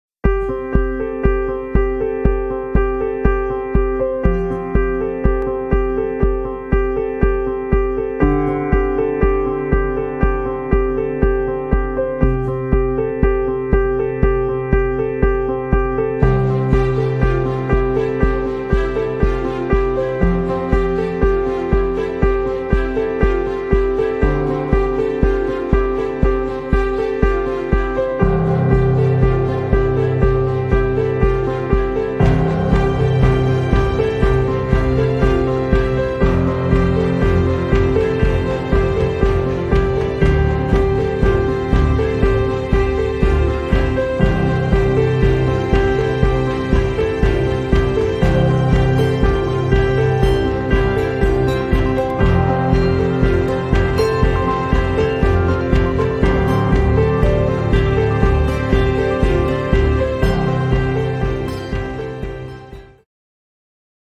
What is flying?
A little animation that shares what we mean by flying in the context of Flying School.